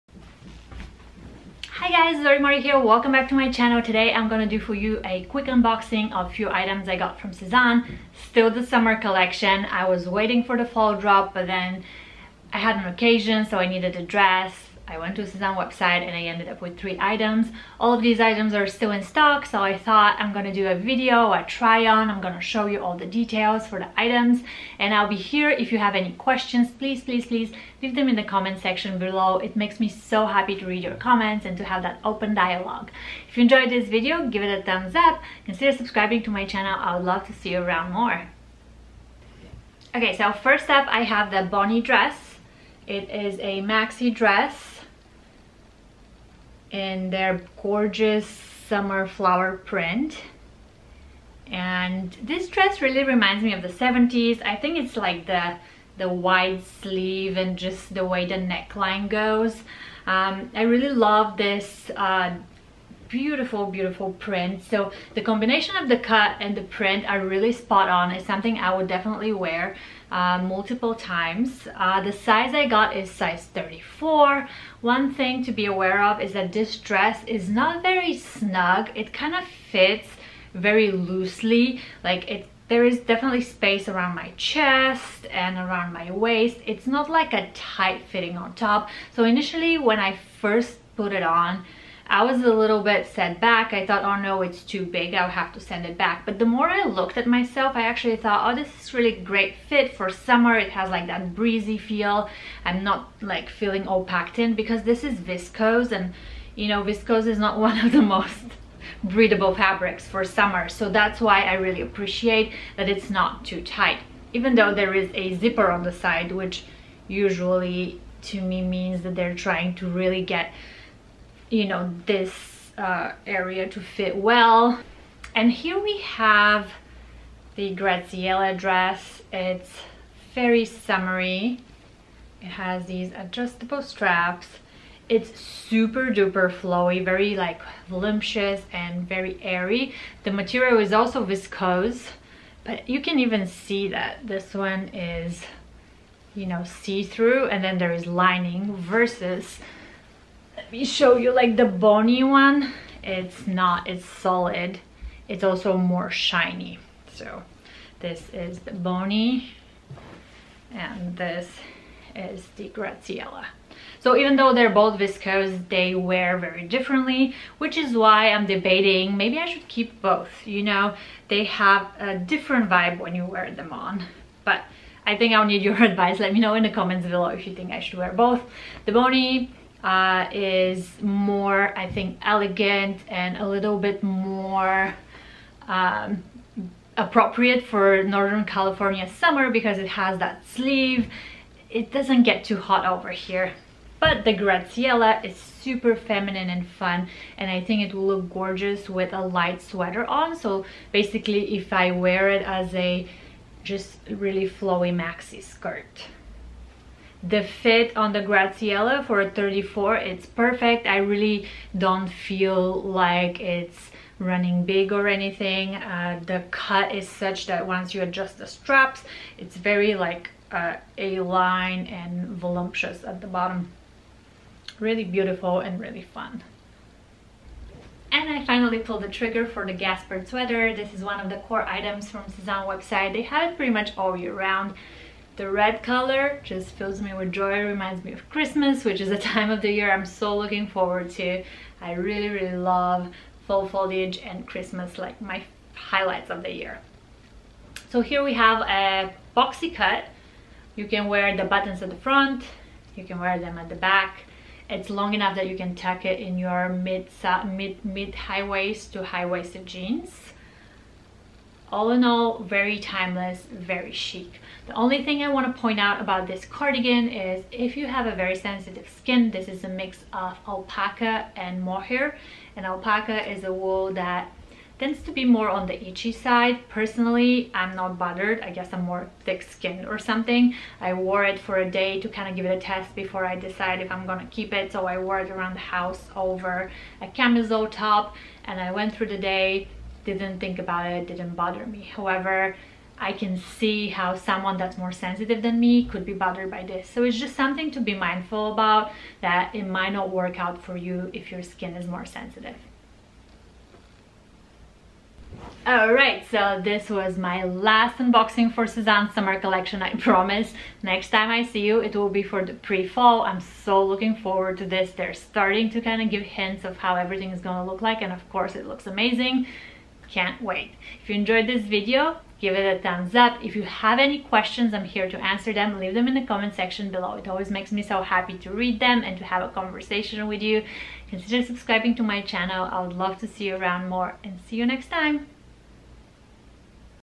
Sezane (last) summer haul & try-on: Graziella Dress, Bonnie Dress & Gaspard Jumper
Oops I did it again:) I had an occasion to shop for 2 weeks ago and I landed on Sezane’s website for one last shopping haul of their summer 2020 collection. I got Graziella Dress, Bonnie Dress & Gaspard Jumper

During the unboxing I’m wearing:
Madewell Smocked Button-Up Top in French Daisies

Look 1:
Sezane Graziella Dress

Look 2:
Sezane Bonnie Dress

Madewell Sweater